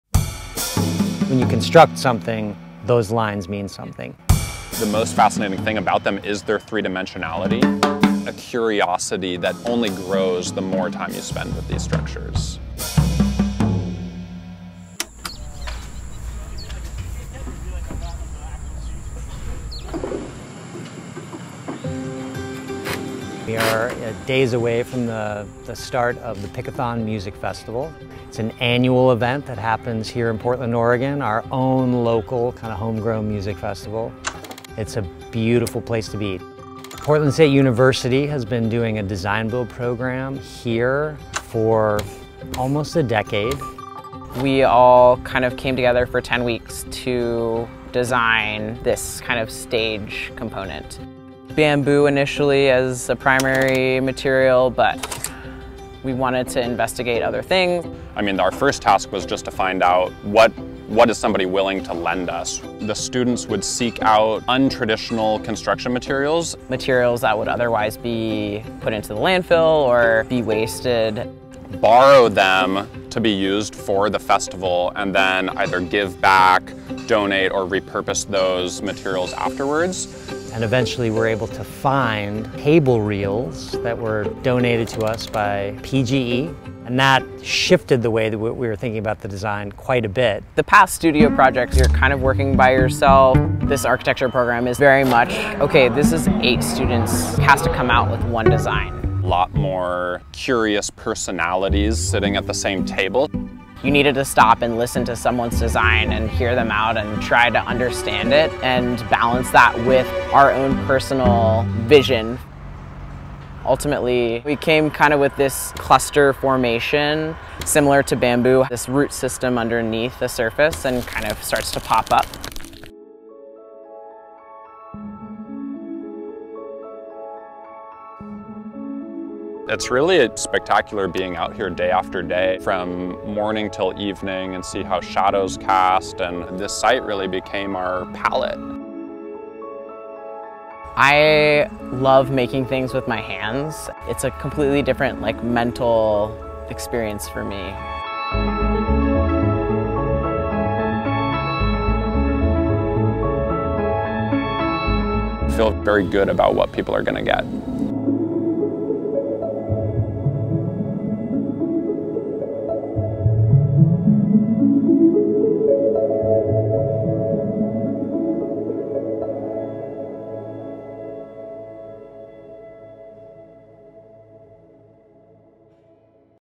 PSU Architecture: Designing the Cherry Hill Stage at Pickathon
As the Pickathon music festival returned from its two-year hiatus, students and faculty at Portland State University School of Architecture created a brand-new, temporary performance venue from reused building materials at the beloved Oregon music festival, which happened August 4 to 7, 2022 in Happy Valley, Oregon.